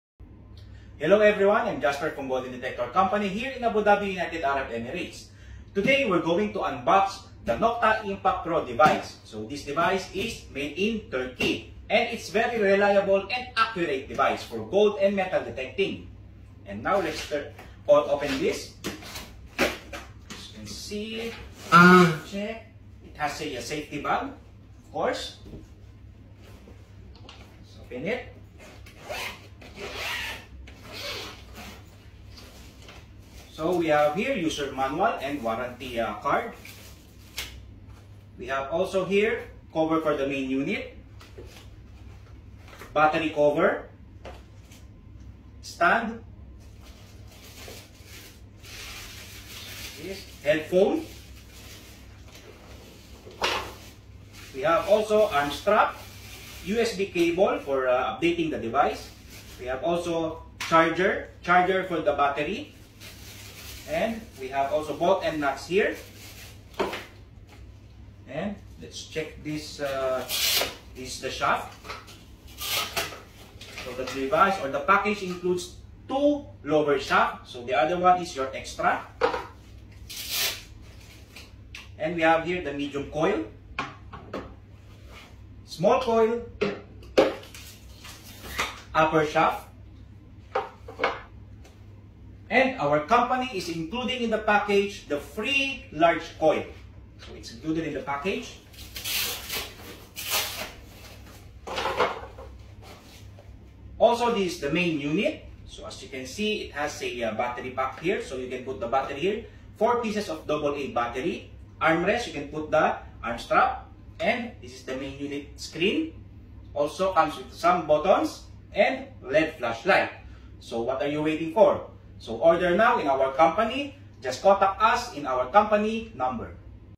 Unboxing Nokta Impact Pro metal detector
IMPACT PRO | NOKTA | BEST DETECTOR

     The IMPACT boasts (12) search modes designed to hunt out different targets such as coins, relics and deep treasure caches. It also operates extremely well in different terrains including (but not limited to) wet beach sand and parks, allowing you to search without any further adjustment if you wish.

     Featuring (3) powerful operating frequencies, online firmware update (via PC USB) capability and robust design. If you are looking for depth and advanced discrimination ability, the IMPACT - just like its name – will make a great impact on your future detecting finds.

Impact Pro package
- System Box
- Waterproof DD Search Coil 28x18 cm / 11" x 7" (IM28) & Cover
- Waterproof DD Search Coil 19x10 cm / 7.5" x 4" (IM19) & Cover
- Waterproof DD Search Coil 39x45 cm / 15"x17.5" (IM45) & Cover
- Headphones
- Extra Lower Shaft
- USB Data Cable
- USB Charger & 4 x AA Rechargeable Batteries
- User Manual
- Stand
- Protective Covers
- Carrying Bag

Contact Person: Jasper